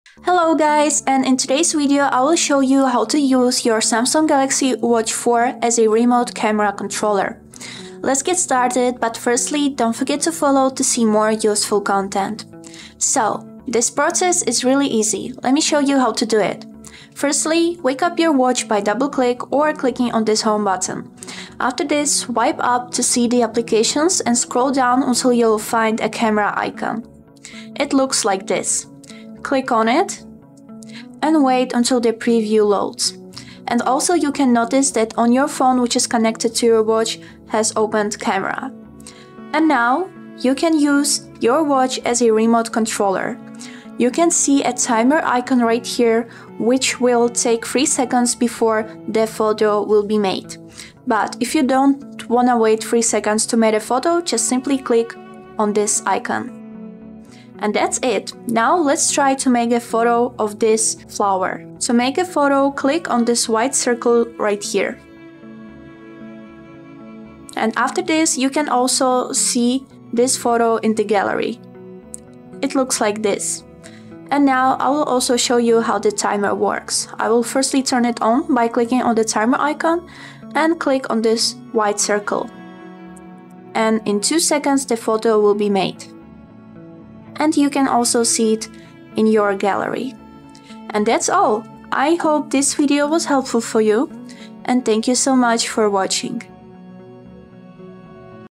How to Take a Photo with Galaxy Watch 4: Remote Camera Magic
Discover the secret to perfect selfies and group shots! Learn how to effortlessly control your phone's camera using your Galaxy Watch 4. This easy-to-follow guide will show you how to quickly set up and use your watch as a remote shutter, unlocking a world of hands-free photography fun. Capture those unforgettable moments with ease!